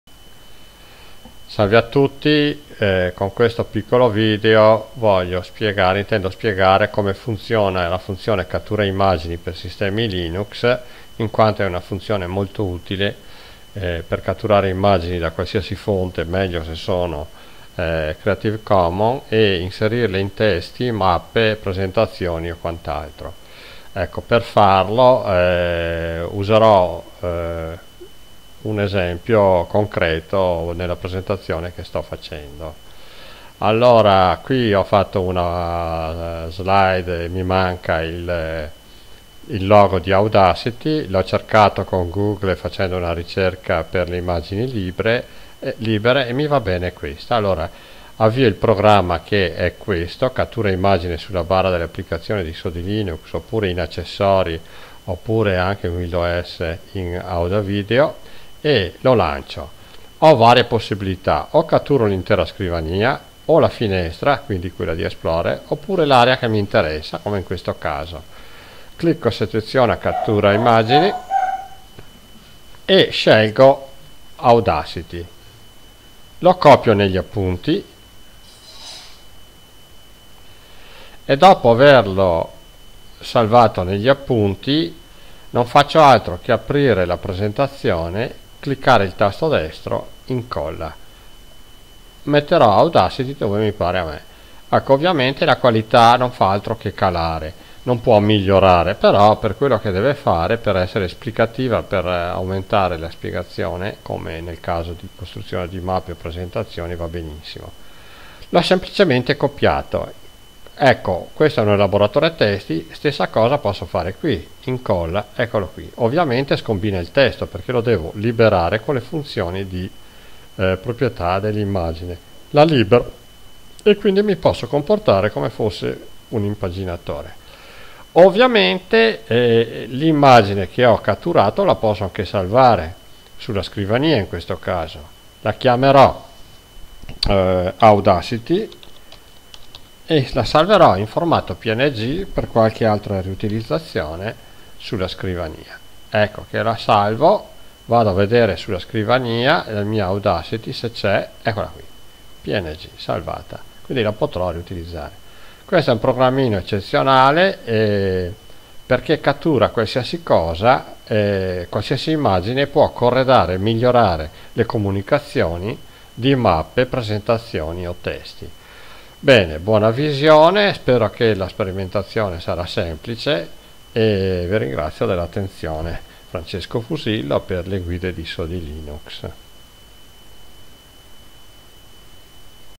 come catturare immagni e corredare una presentazione
Come corredare una risorsa con immagini catturate dallo schermo
con la funzione
cattura immagini